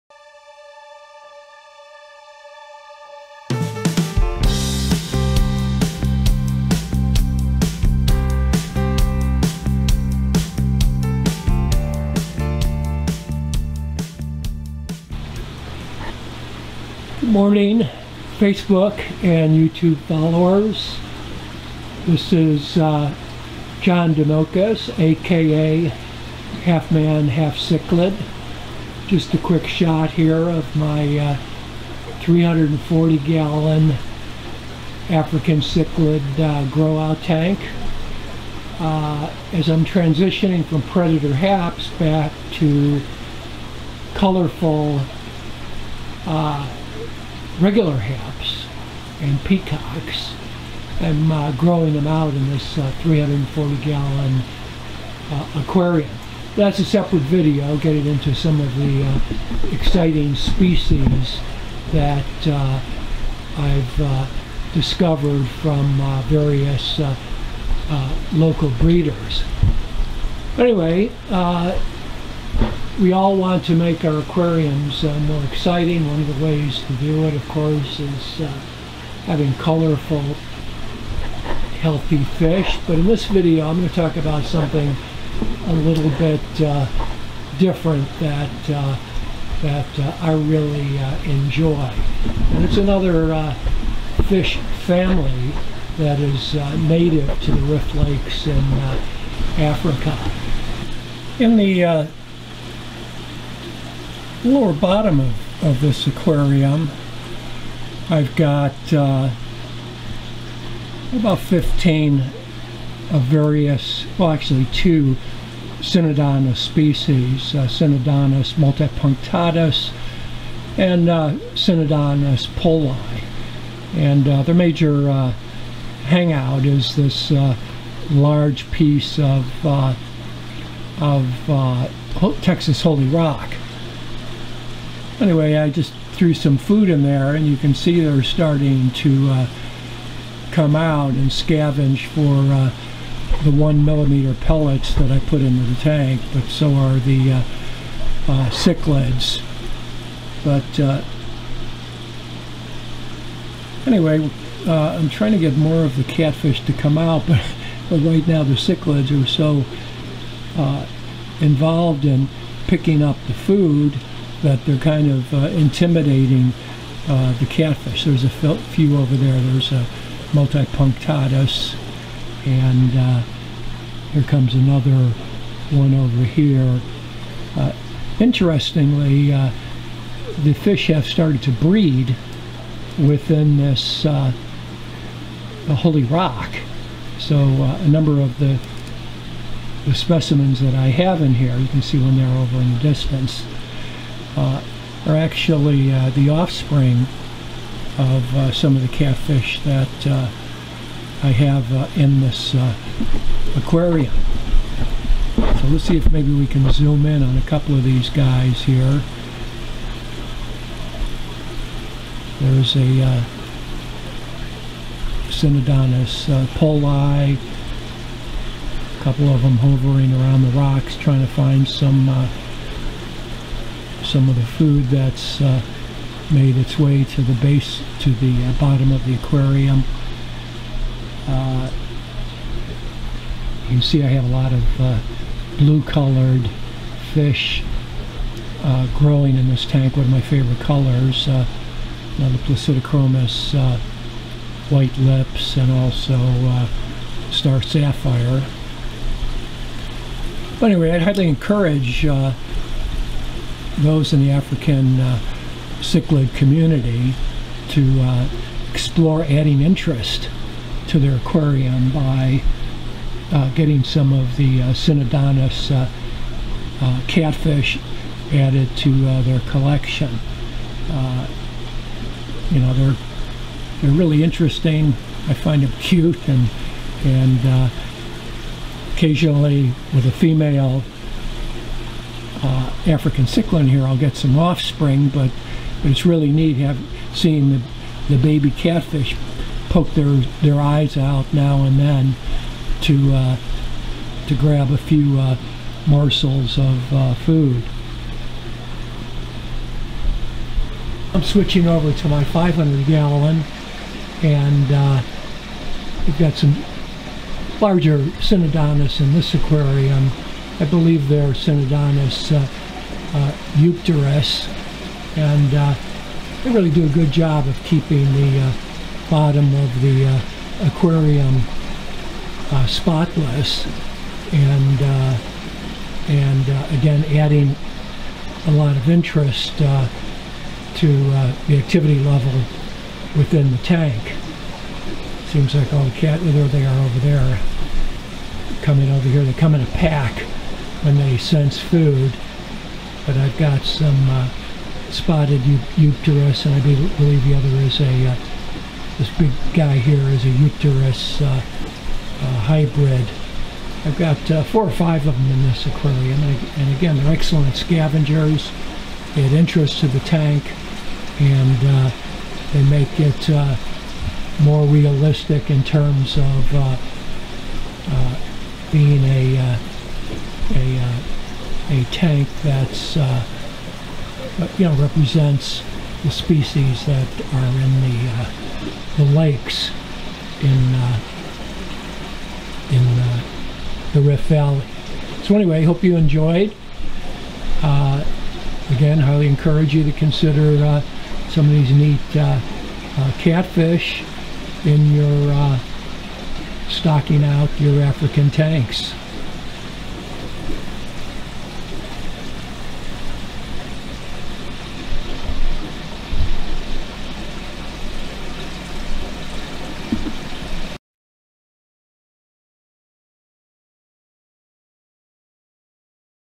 I love Synodontis in African Tanks
Expand the appeal and interest of your Rift Lake fish collection.  Consider adding Synodontis catfish.  Many different species to select from...